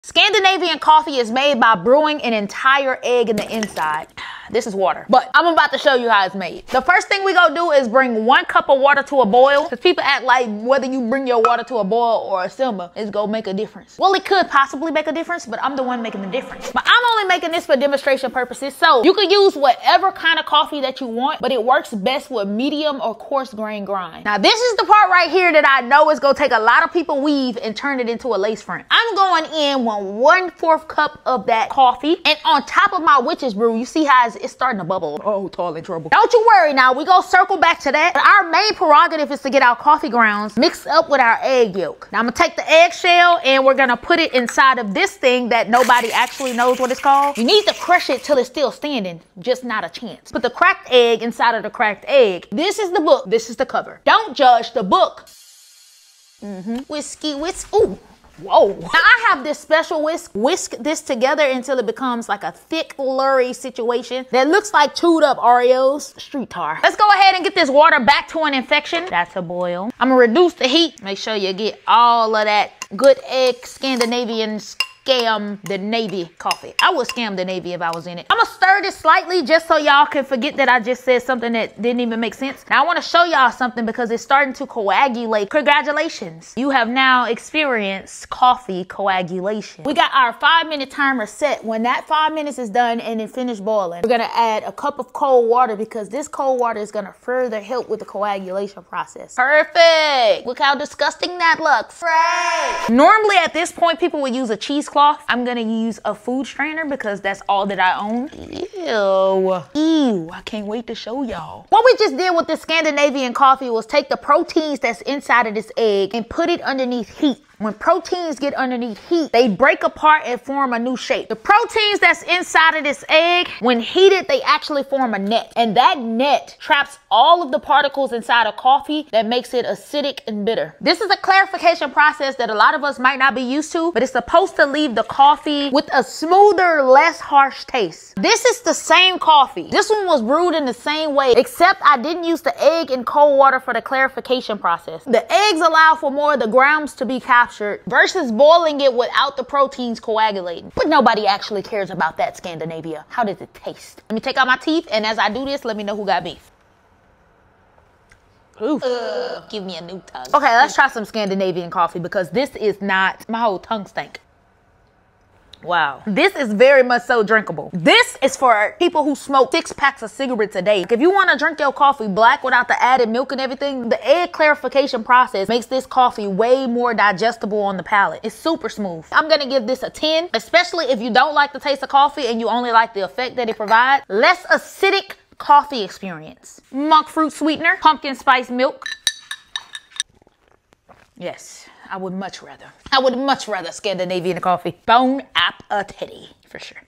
This Freshly Brewed Coffee Has An Entire Egg IN IT!
Freshly Brewed Coffee… with an Entire Egg (Shell Included)?! Let Me Show You!

Have you ever tried freshly brewed coffee with a whole egg cracked inside — shell and all? It might sound wild, but this Scandinavian tradition is a thing. The egg’s proteins work to clarify the coffee, binding to impurities and reducing bitterness, making it smoother and less acidic. Even the shell helps, as it introduces calcium carbonate, neutralizing the coffee’s acids. It’s not just science — it’s a cup of silky, clarified perfection 😌… but some respect on its name 😂😂